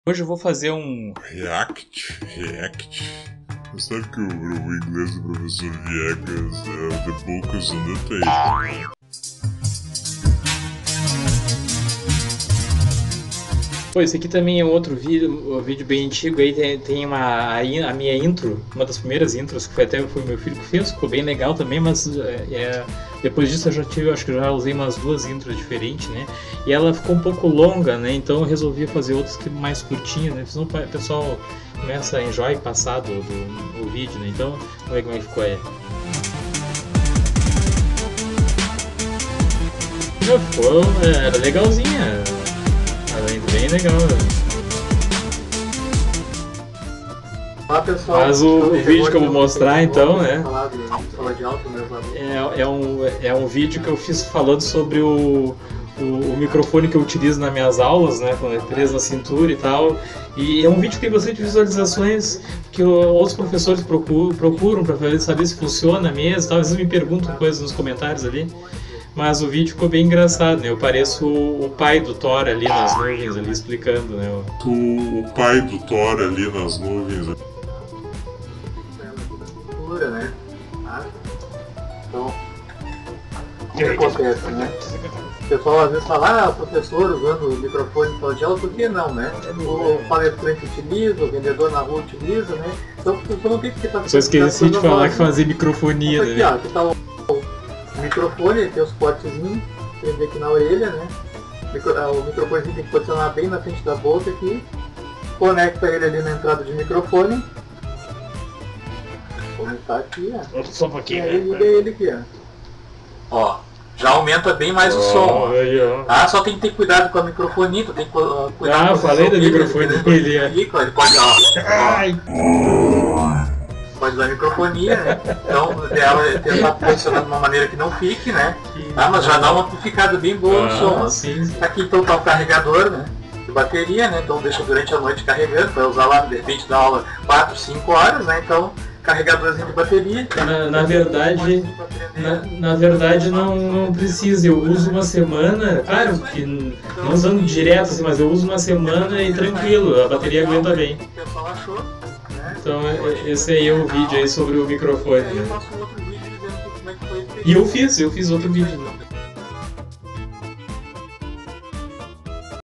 REAGINDO AO MEU VÍDEO MOSTRANDO O MICROFONE PORTÁTIL DE USAR EM SALA DE AULA - PROFESSOR VIÉGAS
Quarto vídeo da série "REACTS". Nesse vídeo estou reagindo aos dois vídeos que fiz sobre o microfone que uso em sala de aula.

Editor e Autor: Profº Sandro Giovani P. Viégas
Editor de vídeo: Camtasia 9
Editores de Imagem: Photoshop CC e Photoscape
Editor de Áudio: Audacity